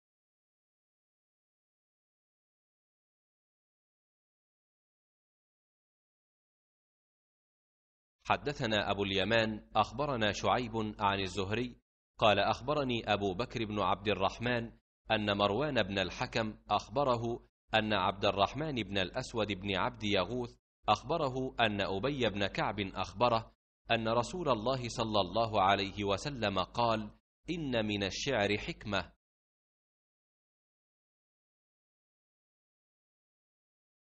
صحيح البخارى - كتاب الأدب - 6145 - باب ما يجوز من الشعر والرجز والحداء وما يكره منه
حدثنا أبو اليمان، أخبرنا شعيب، عن الزهري، قال أخبرني أبو بكر بن عبد الرحمن، أن مروان بن الحكم، أخبره أن عبد الرحمن بن الأسود بن عبد يغوث أخبره أن أبى بن كعب أخبره أن رسول الله صلى الله عليه وسلم قال ‏  "‏ إن من الشعر حكمة ‏"  ‏‏.‏